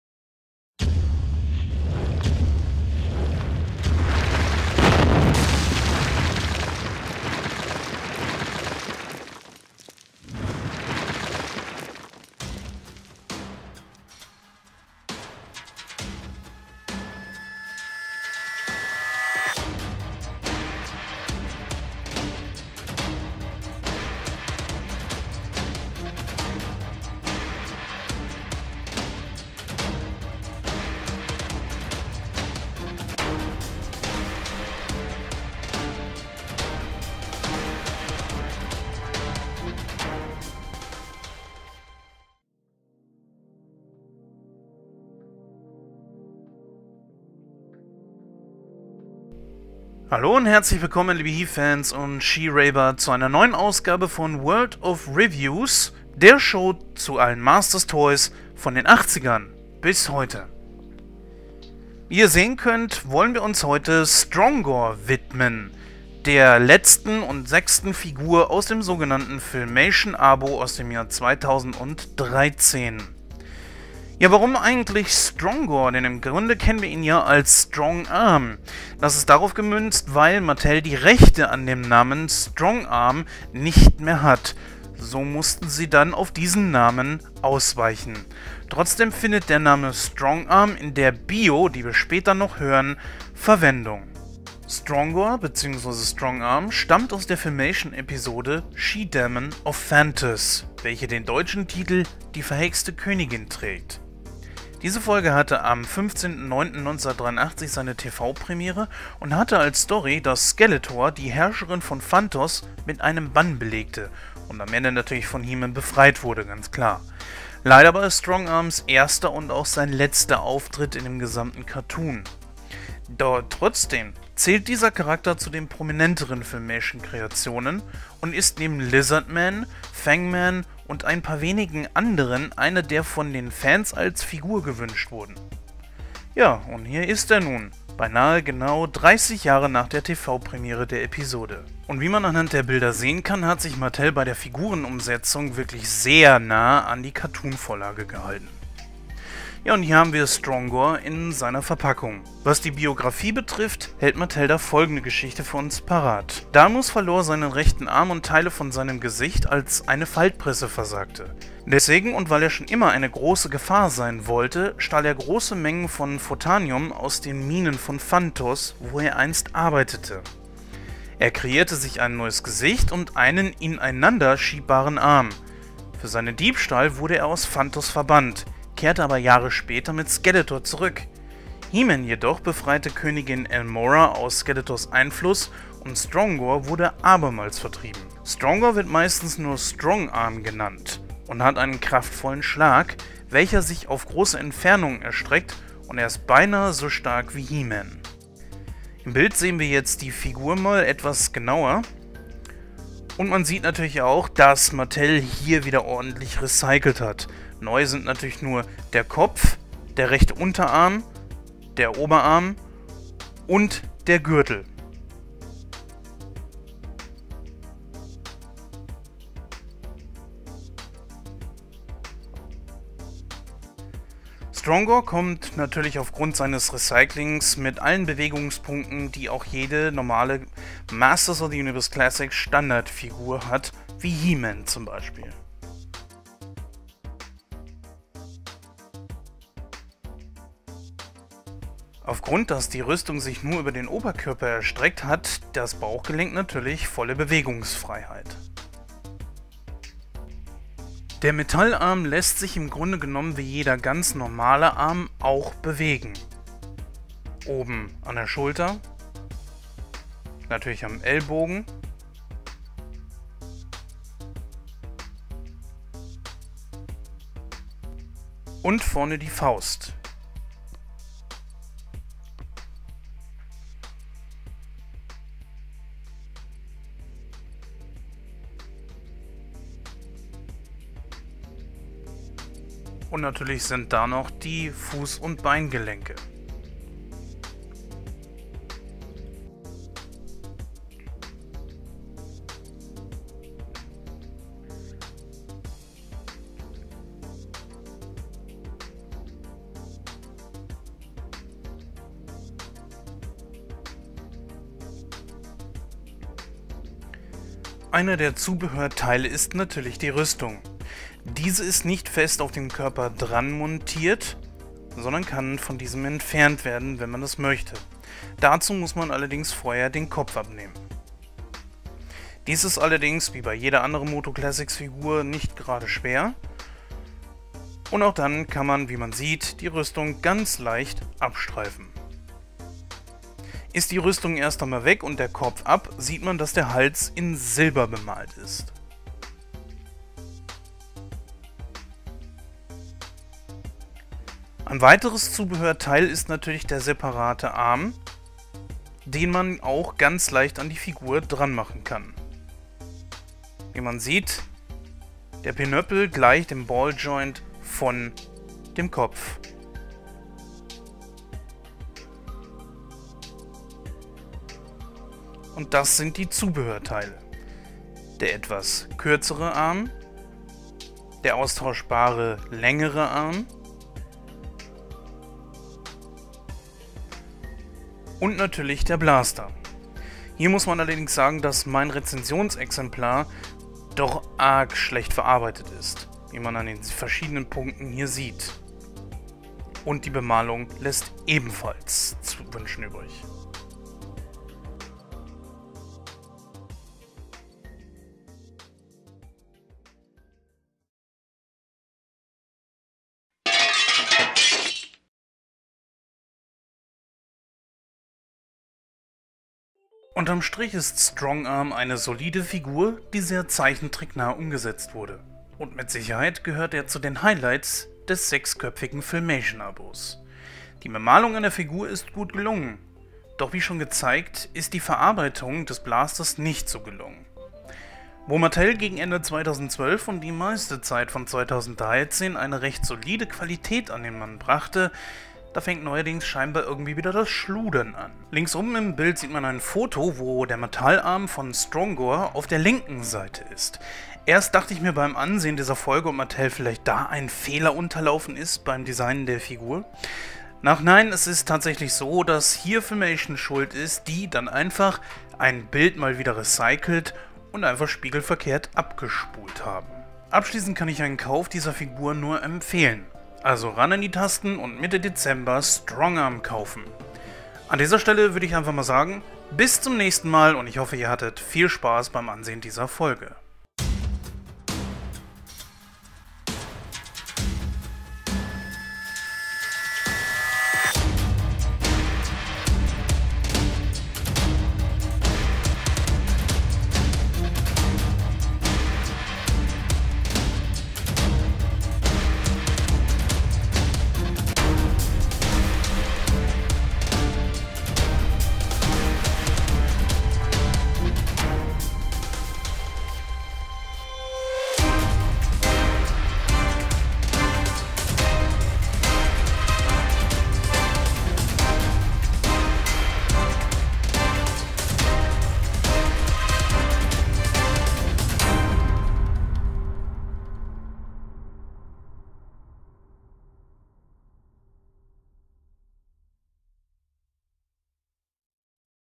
World of Reviews: Strong Or/Strong Arm (MotU Classics)
Auf das Jahr genau sind nun 30 Jahre vergangen seitdem die einzige Episode mit Strong-Arm das erste mal über den Bildschirm flimmerte. Damals dachten viele dass es diesen Charakter vielleicht auch als Figur geben wird. Doch dass man dafür 3 Dekaden warten muss hätte sicher keiner von uns als Kind gedacht. Doch ob auch wirklich gut wird/ist was lange währt, das erfahrt ihr im Review.

Und nun viel Spaß beim ansehen…